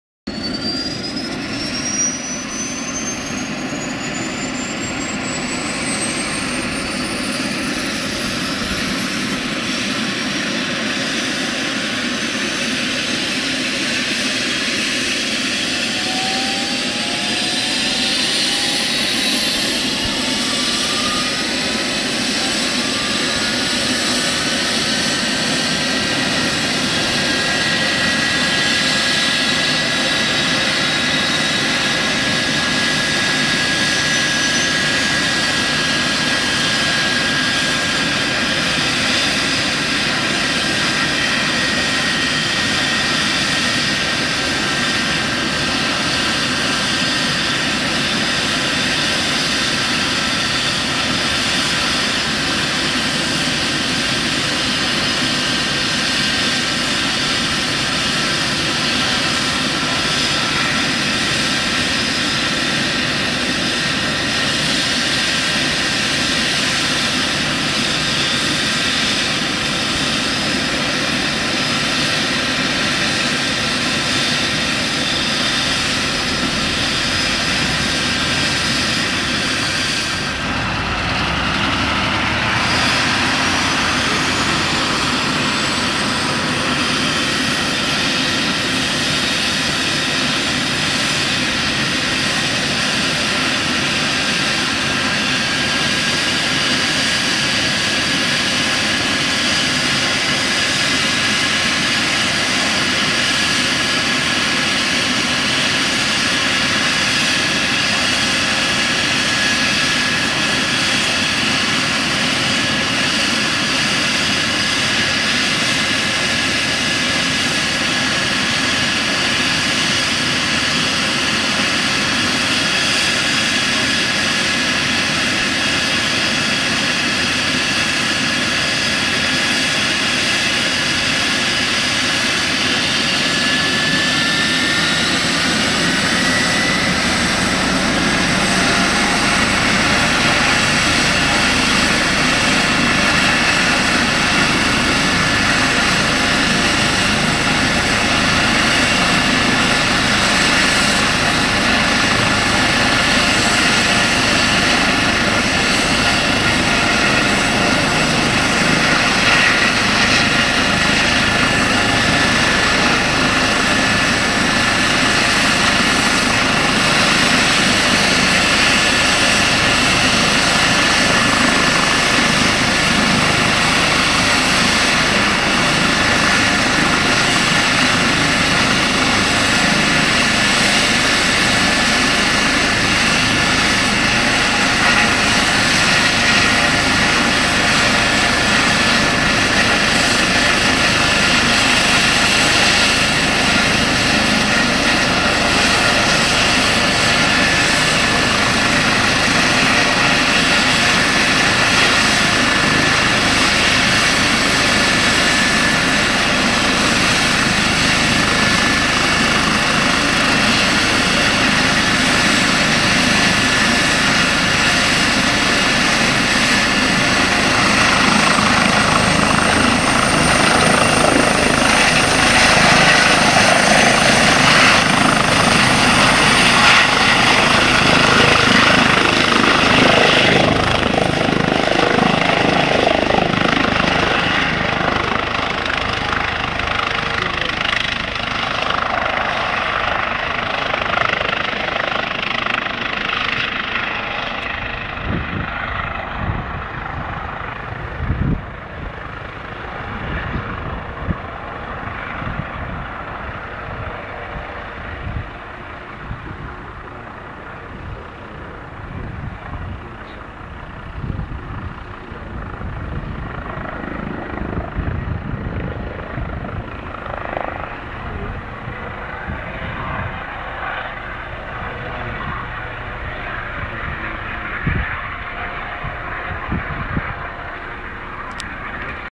Décollage SAMU Lorraine
F-GNLO au décollage depuis l'aérodrôme de Nancy-Essey